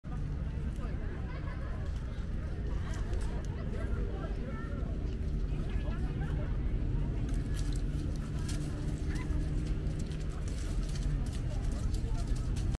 2023年6月撮影 奈良公園 シカの謎行動（Unusual behavior of deer in Nara Park.）
奈良公園で見かけた鹿の珍しい行動を撮影しました。

住所：奈良県奈良市登大路町30

近鉄奈良駅、シカ、しか、鹿、名勝、奇行、授乳？

Unusual behavior of deer in Nara Park.